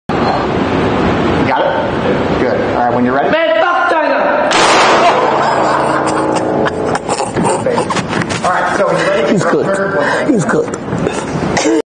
Ray trains to fight the CCP
The top comment of the post was: FYI, Ray (or rayasianboy) is Kai's Taiwanese friend who's leaving for mandatory military service soon-ish.